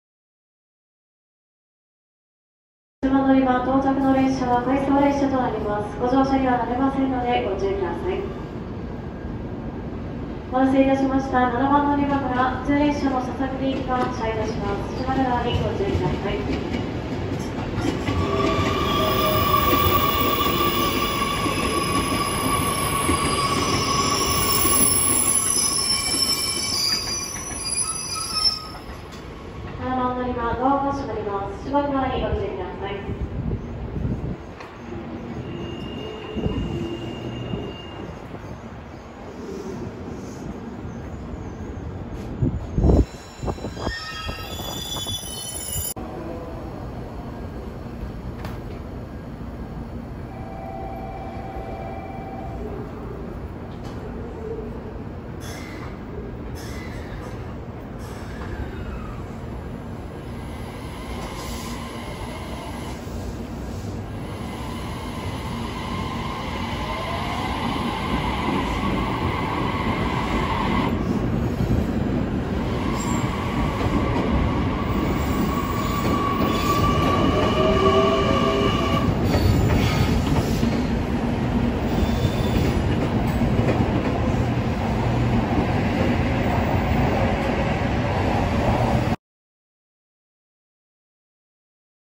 ８１７系ＶＧ１０９＋ＶＧ１１０２ 福北ゆたか線普通博多行５６２５Ｈ終点、博多駅到着ー回送発車
２０１９年１２両２７日撮影

篠栗行き（福北ゆたか線）の４両編成の列車で唯一、ワンマン運転ではなく、車掌が乗務する列車です。